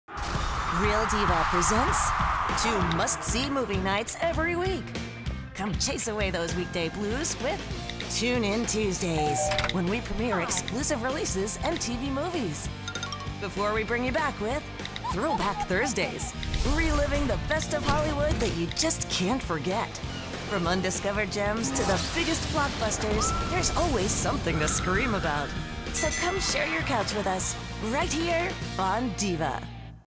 DIVA Loves Movies TuneInTuesdays and ThrowBackThursdays Stunt Promo
NBC Universal Asia - DIVA
DIVA Love Movies Stunt
Producer : Chris Leow
Sound Design - Audio Mix : Suite Sound